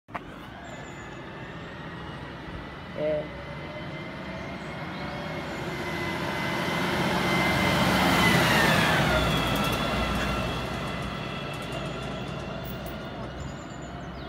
oZo/Panda Renault Driving by in Sandymount
A little clip of this lorry I seen in Sandymount last week, enjoy